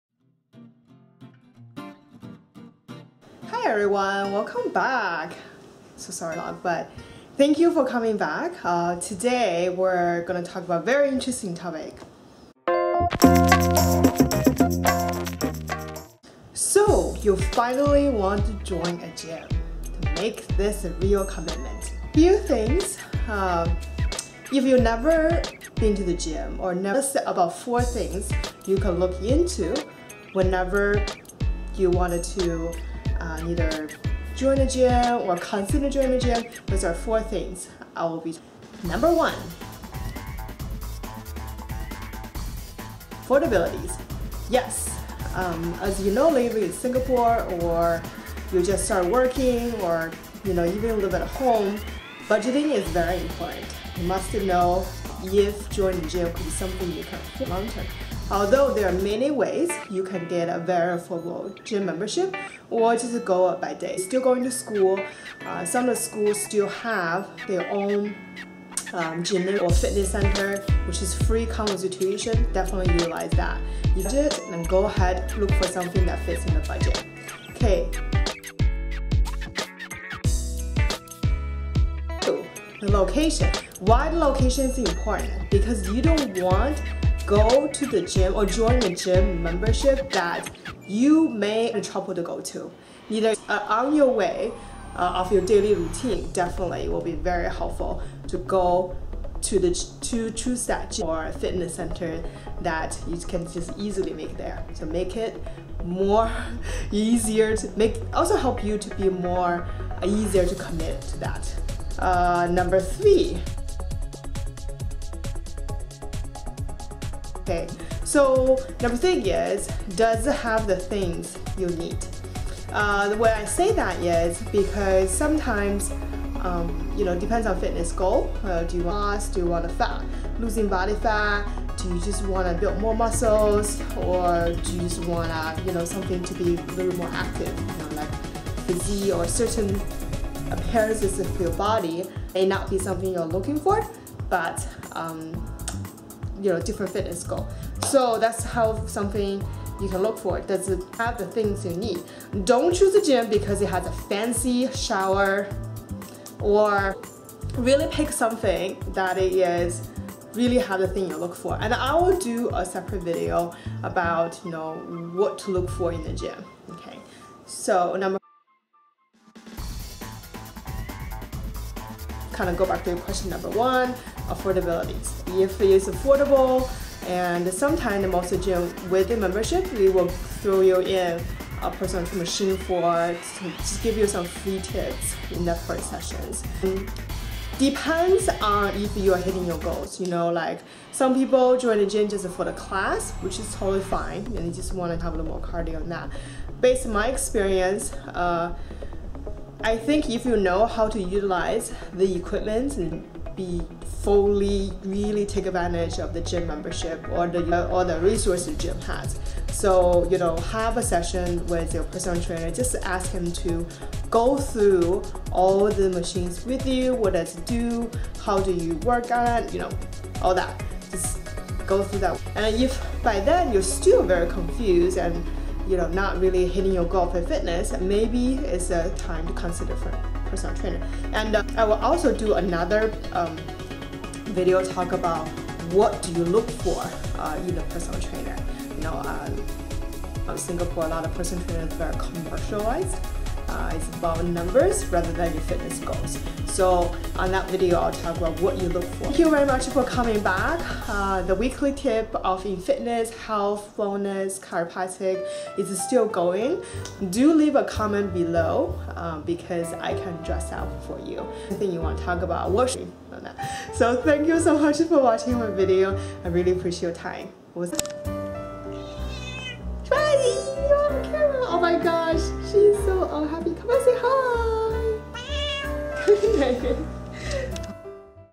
Vlog #2 how to choose the right gym just for you!
This video is about how to choose a gym. 4 tips get you started!